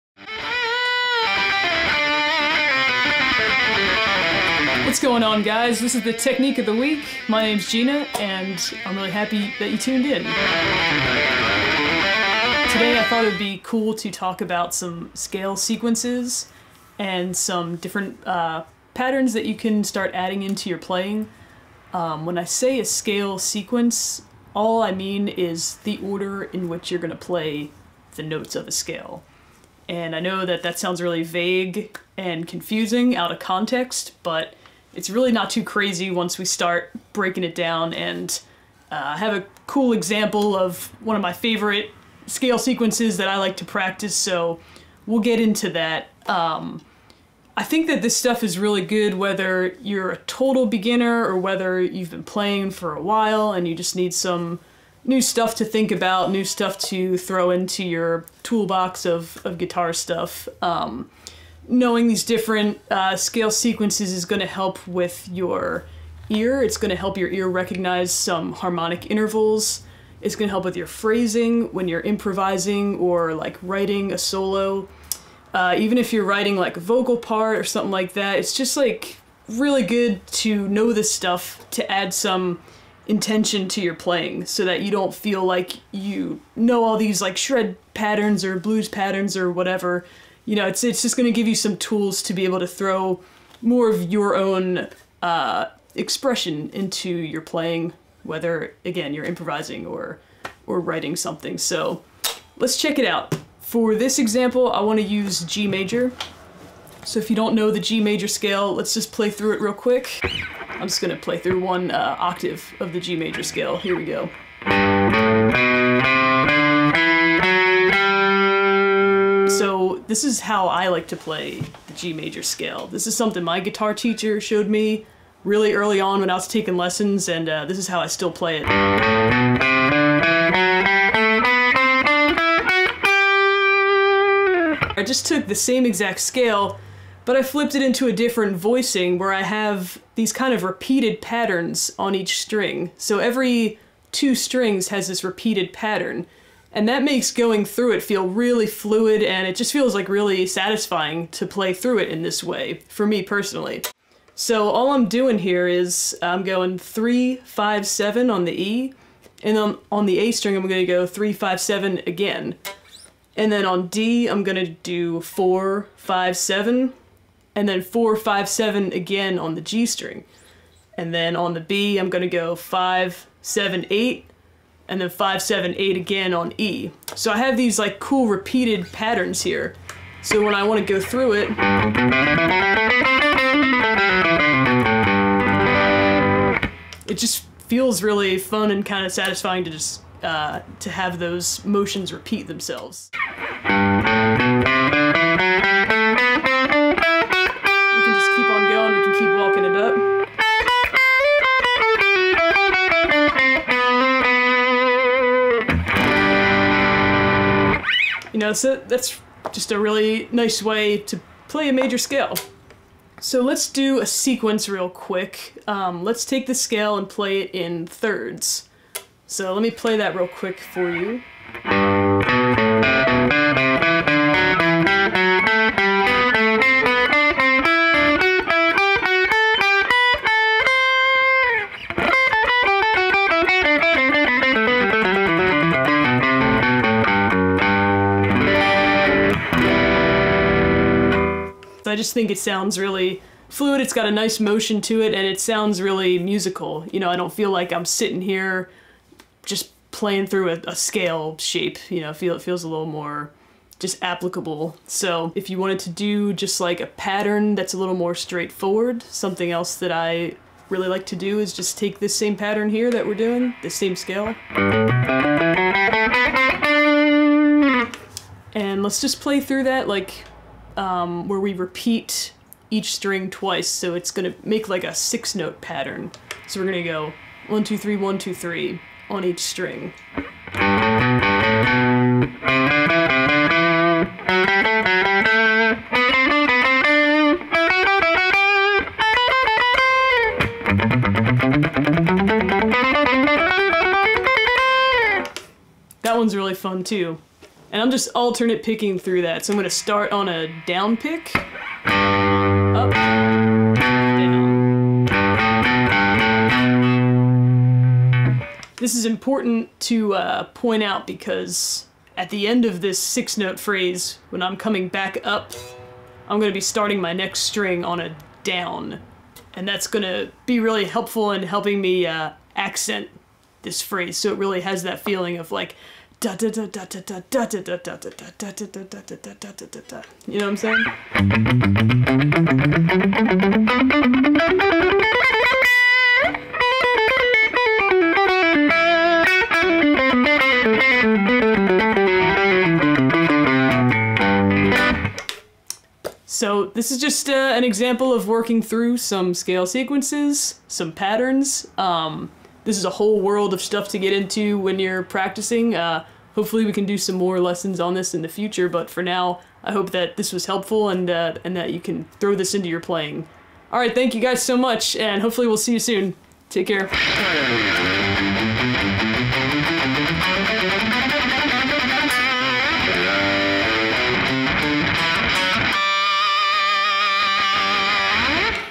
Gina Gleason Explains Scale Sequences | Technique of the Week | Fender
We teamed up with Fender artists to share their favorite riffs, techniques and tips. Gina Gleason from Baroness teaches how to add cool new patterns to your playing by utilizing scale sequences; a repeated pattern of specific notes.